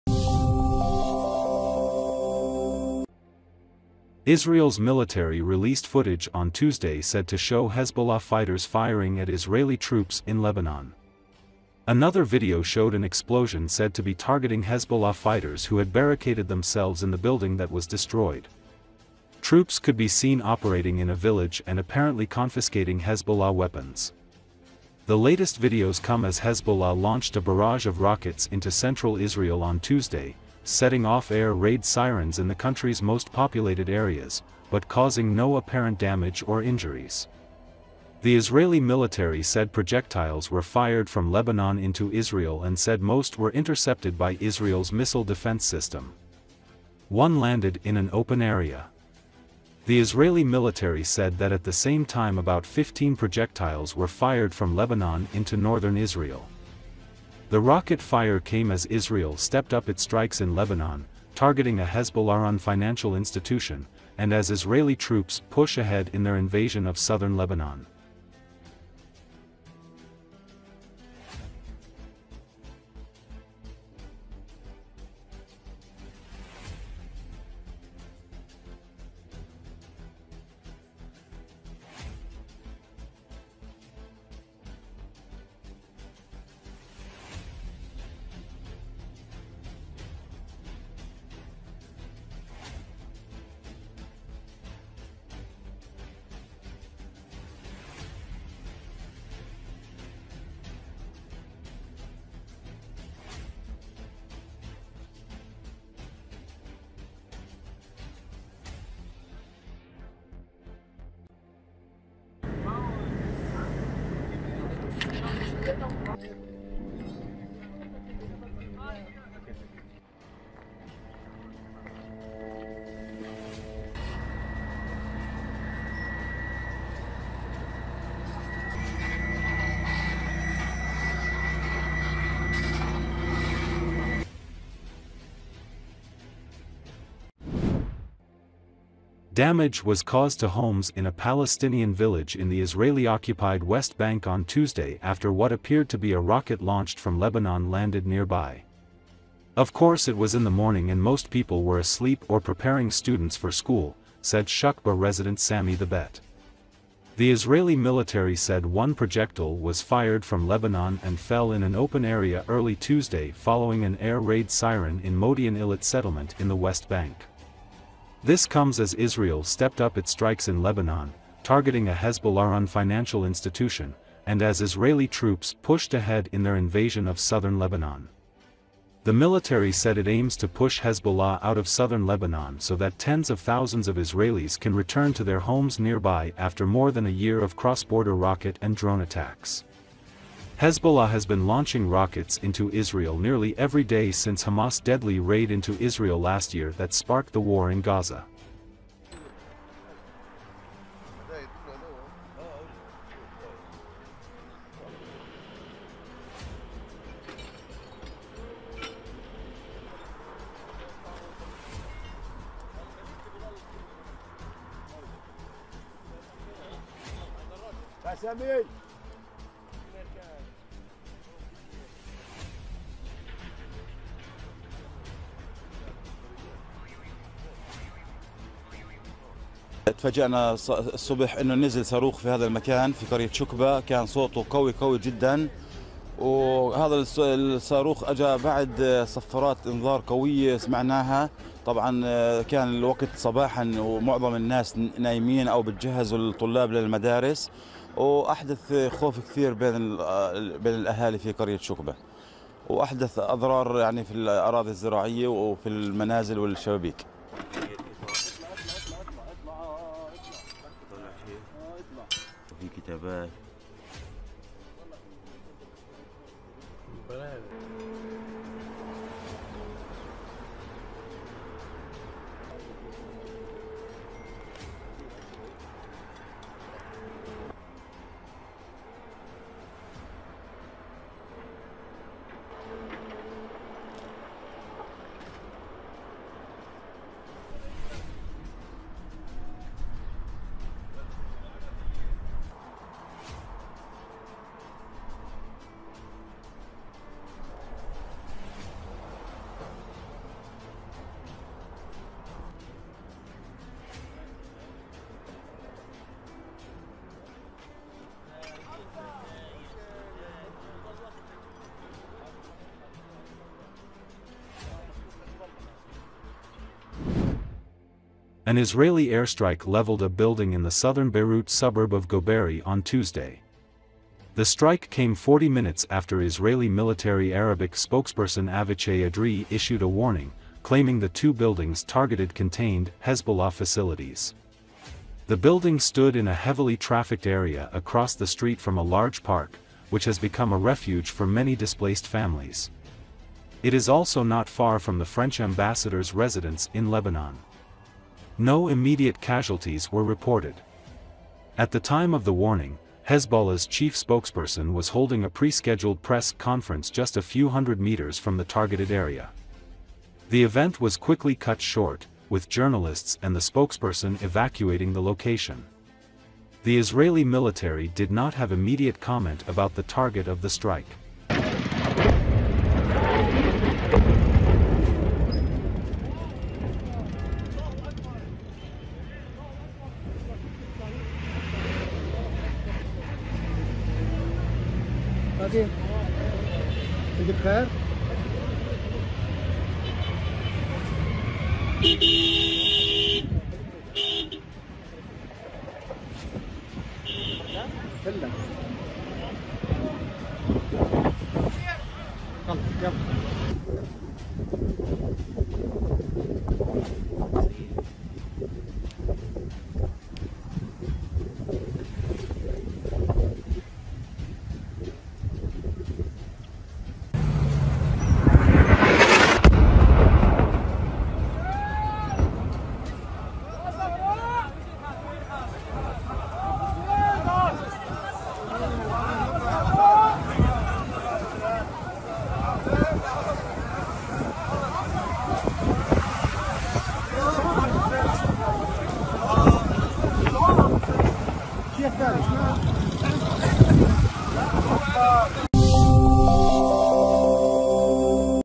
Israeli military releases footage said to show operations in Lebanon
Israel’s military released footage on Tuesday said to show Hezbollah fighters firing at Israeli troops in Lebanon. Another video showed an explosion said to be targeting Hezbollah fighters who had "barricaded themselves in the building" that was destroyed. Troops could be seen operating in a village and apparently confiscating Hezbollah weapons. The latest videos come as Hezbollah launched a barrage of rockets into central Israel on Tuesday, setting off air raid sirens in the country’s most populated areas, but causing no apparent damage or injuries. The Israeli military said five projectiles were fired from Lebanon into Israel and said most were intercepted by Israel’s missile defense system. One landed in an open area. The Israeli military said that at the same time about 15 projectiles were fired from Lebanon into northern Israel. The rocket fire came as Israel stepped up its strikes in Lebanon, targeting a Hezbollah-run financial institution, and as Israeli troops push ahead in their invasion of southern Lebanon.